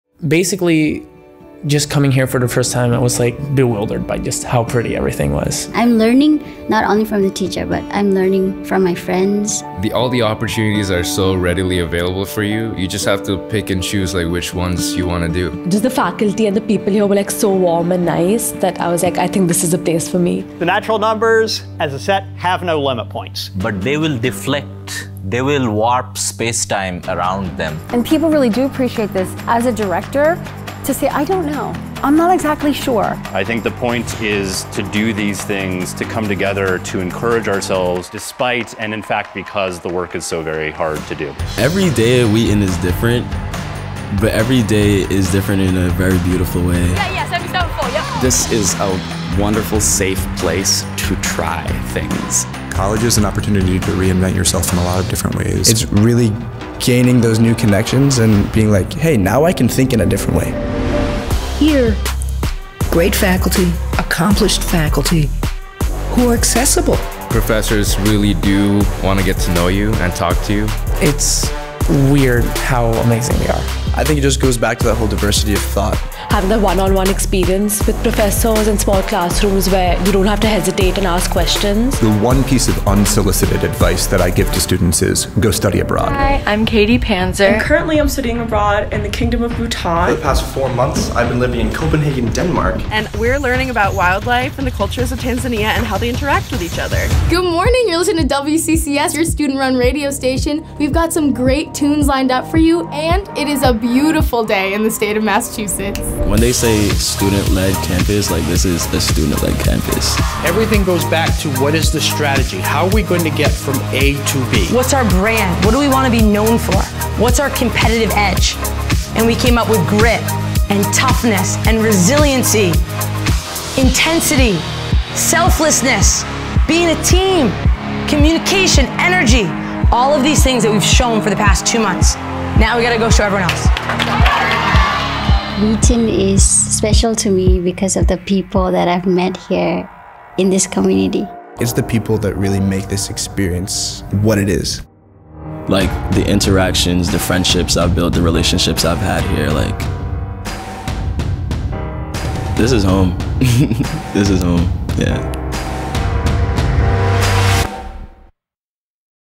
Wheaton: This is Home
Wheaton is a place where we root for each other, support one another, and cheer one another’s successes. Wheaton is home.  This video was filmed, directed, and produced by Film and New Media Studies major, Ben Danielson.